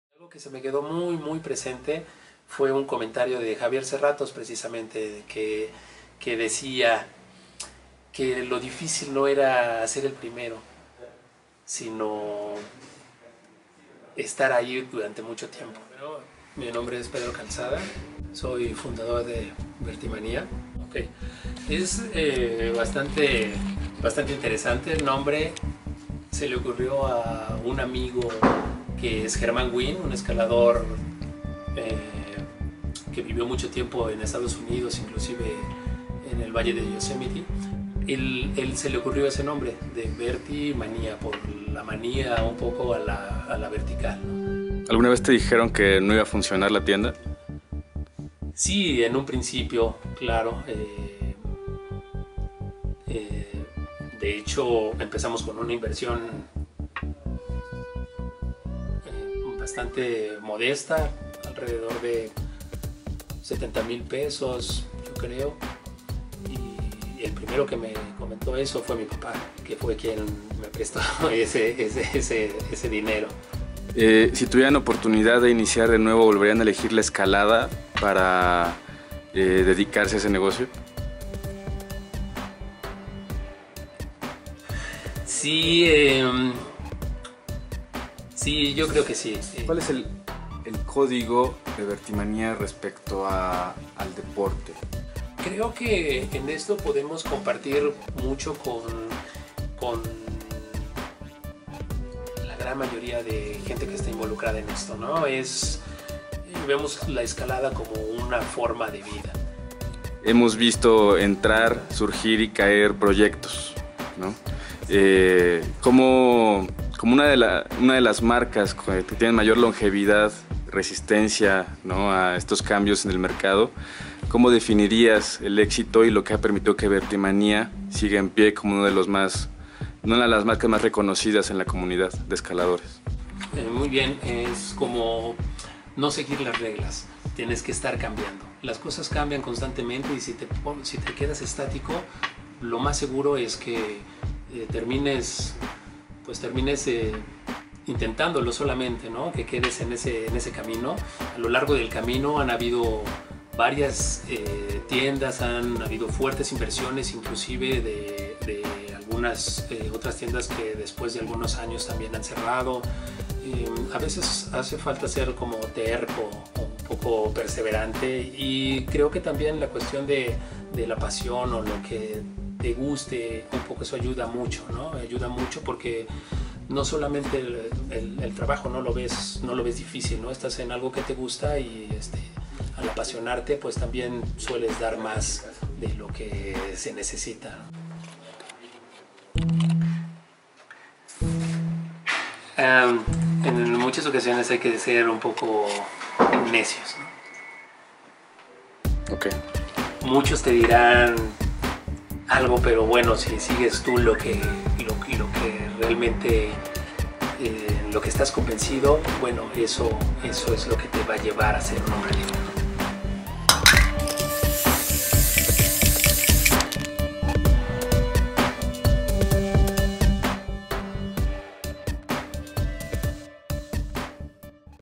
Vertimanía 20 años. Entrevista Pedro Calzada
Vertimanía cumple 20 años de existir en México. ¿Qué ha significa para su fundador la escalada? ¿Qué es para Vertimanía un hombre libre?